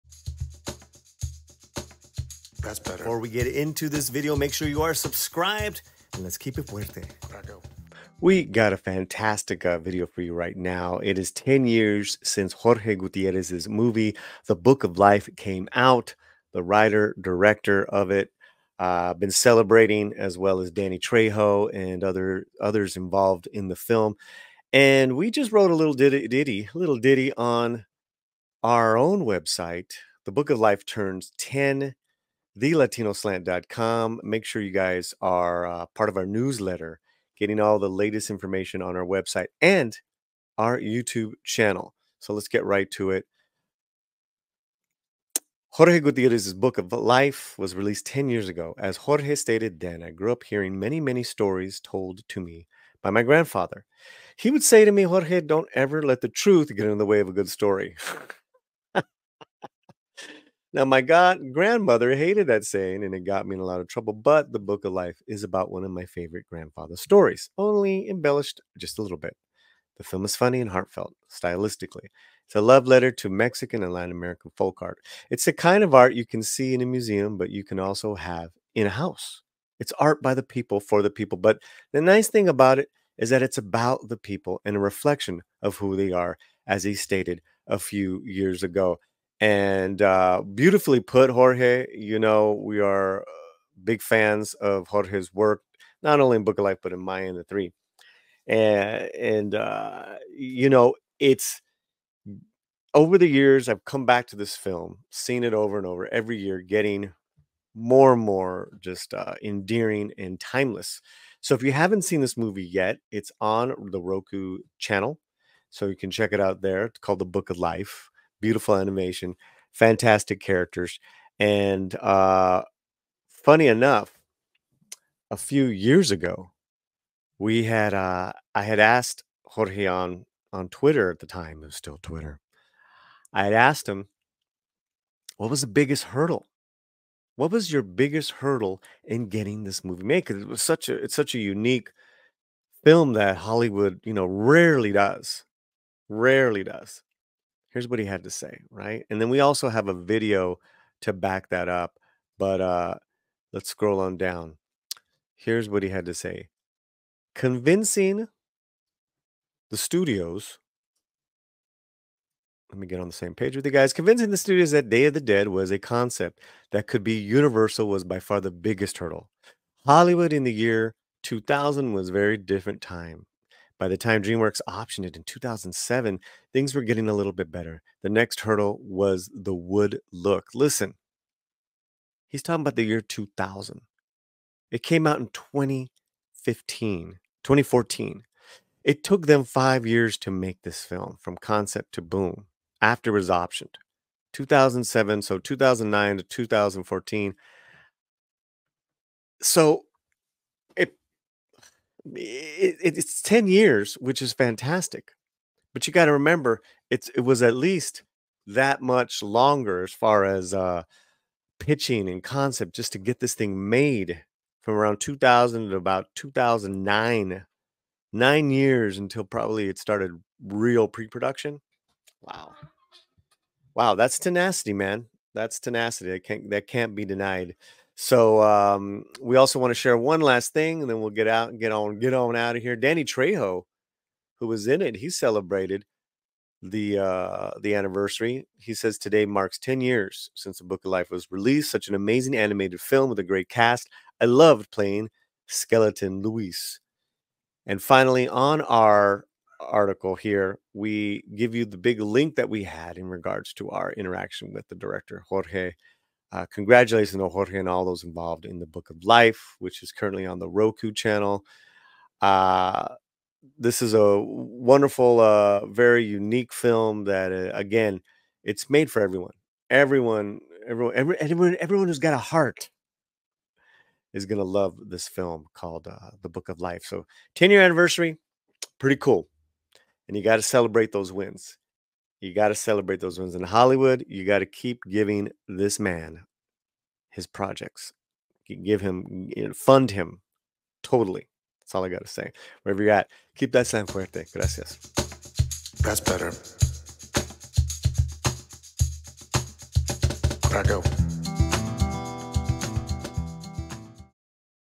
"The Book of Life," Film Turns 10!
❤️ The Book of Life film celebrates its tenth year since being released. Paulie sings its praises!

💪🏽 Want stories that are new and exciting, bold and entertaining? Consider supporting our first fantasy, action novella,

"Where ever you're at, keep your slant fuerte!"